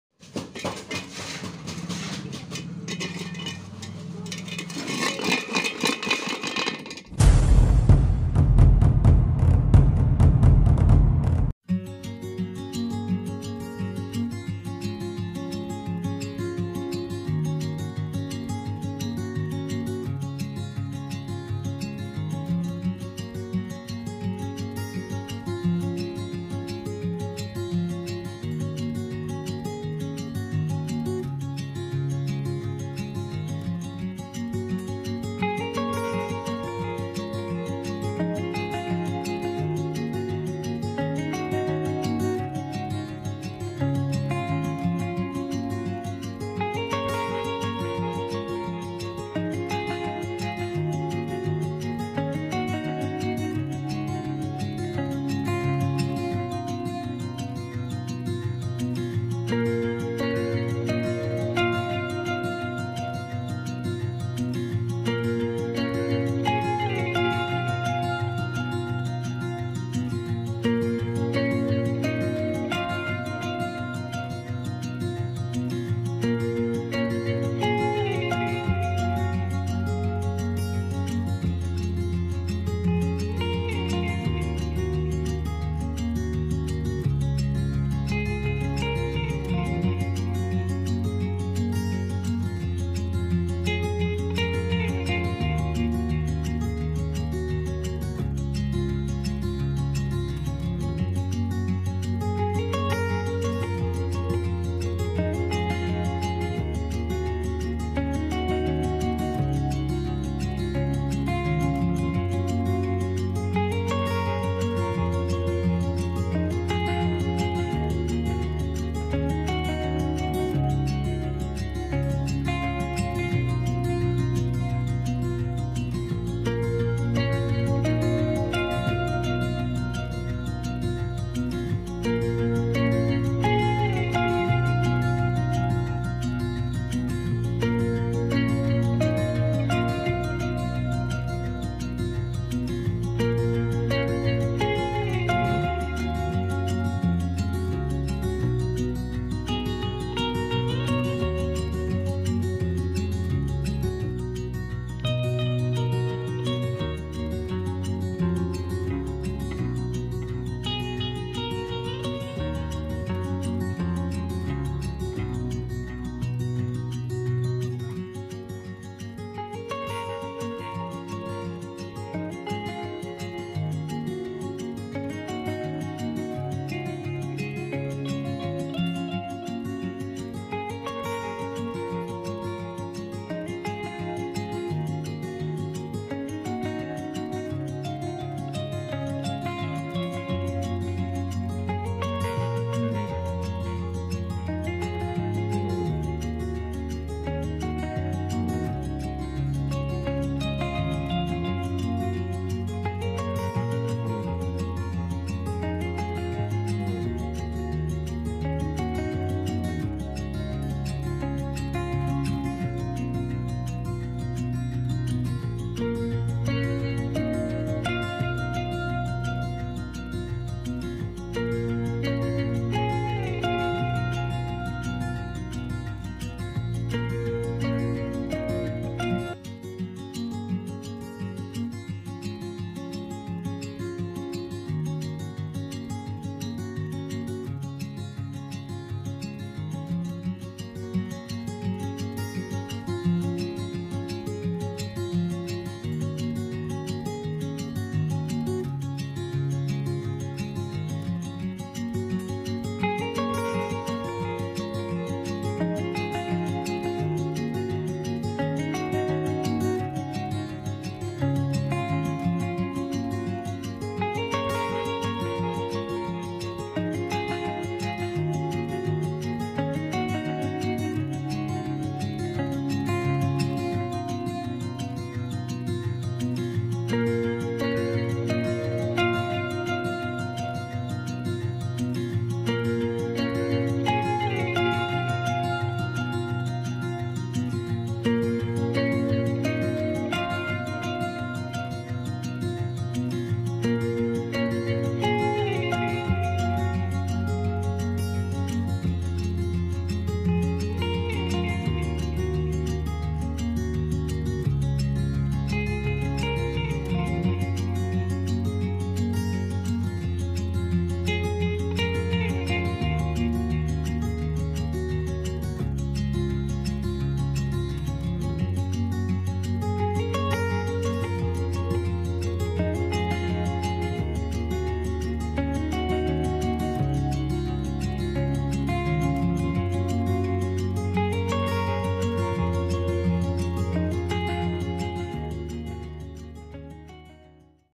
Round Pipe Bending ideas || Diy Round Tube Bending Tool
Welcome To The Fab2ku Family.

Guy's..Let's make a bender for bending a round tube.